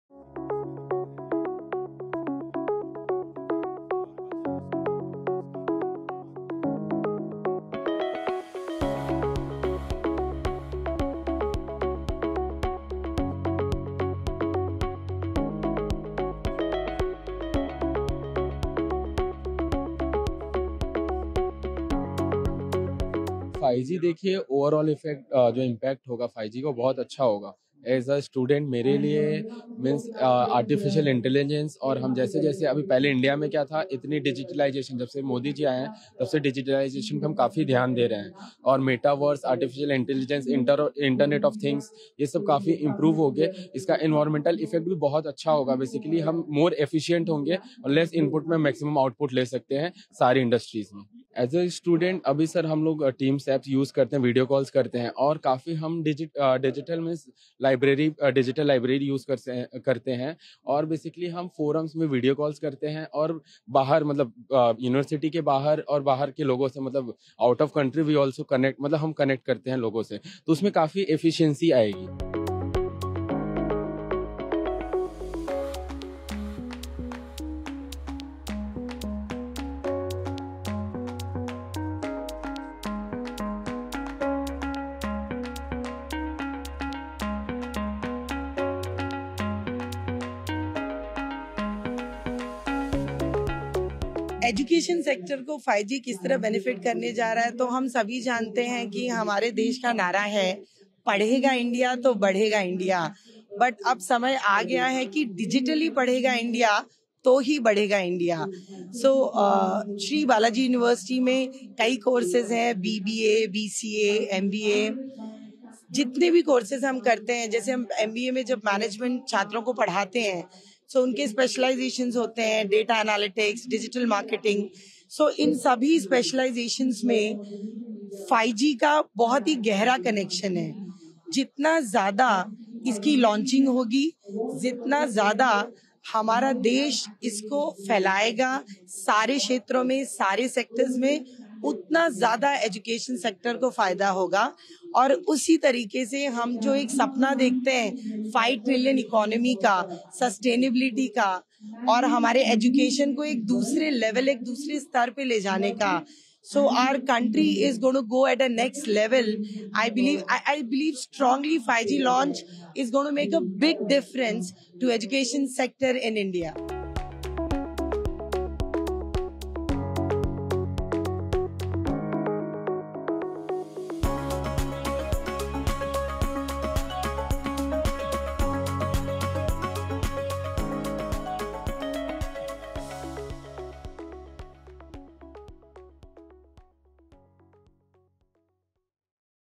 India Mobile Congress 2022 Insights Captured by ANI featuring SBUP Faculty and Student
Insights captured by ANI (PMO) of Associate Professor Dr. Archana Singh, Sri Balaji University, Pune and
Student Manager Vineet Tiwari of Balaji Institute of International Business,
at Pragati Maidan, New Delhi,
5G launch with Honorable Prime Minister Narendra Modiji.